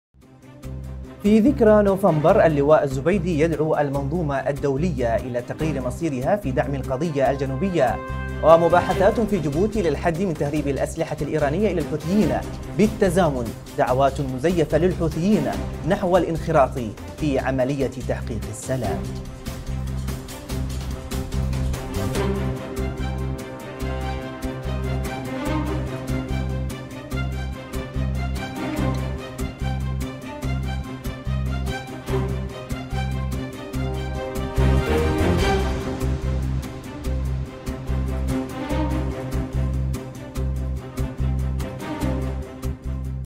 مقدمة المحور_السياسي
في ذكرى نوفمبر اللواء الزبيدي يدعو المنظومة الدولية إلى تقرير مصيرها في دعم القضية الجنوبية ومباحثاتٌ في جيبوتي للحد من تهريب الأسلحة الإيرانية إلى الحوثيين بالتزامن دعوات مزيفة للحوثيين، نحو الانخراط في عملية تحقيق السلام